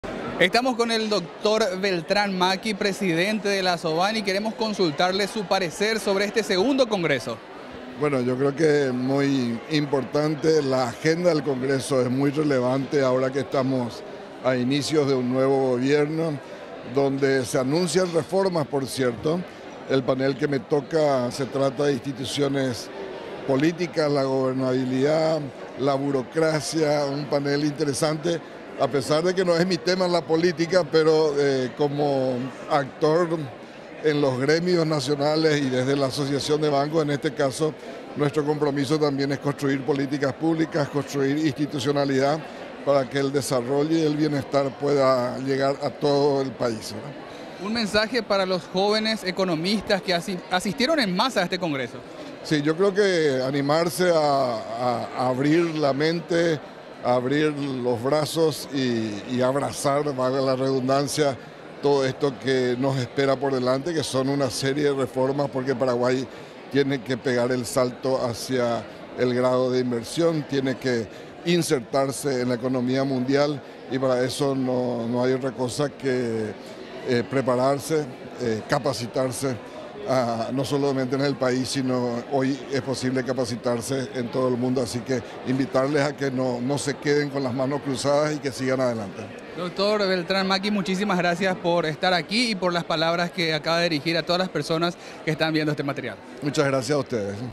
Entrevista. Dr Beltrán Macchi Salím
Segundo Congreso Paraguayo de Investigadores, Productores y Hacedores de Políticas Económicas